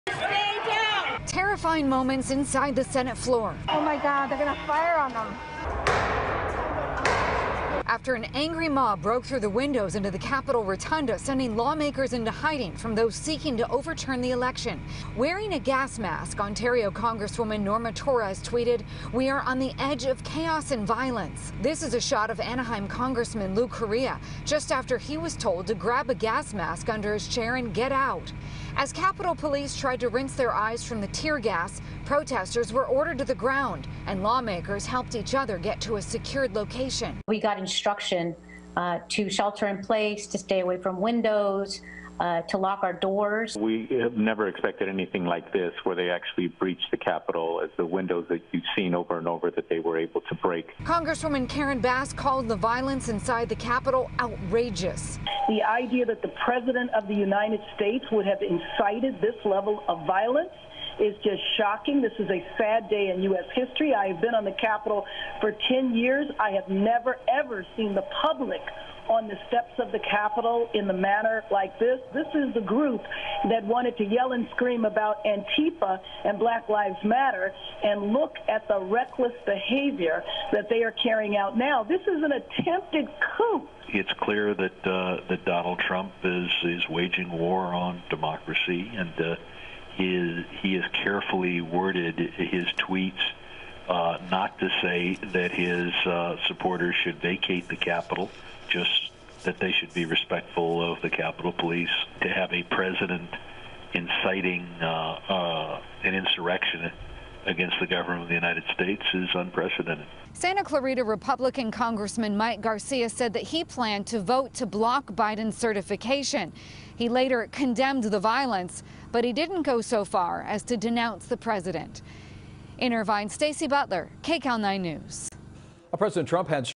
'You Could Smell The Tear Gas': OC Reps Describe Being At U.S. Capitol During Storm
Rep. Lou Correa, D-Santa Ana, was on the House floor Wednesday when the mob that stormed the Capitol tried to barge in, but he said many of his Republican and Democratic colleagues joined together. Correa said it was "just like a movie."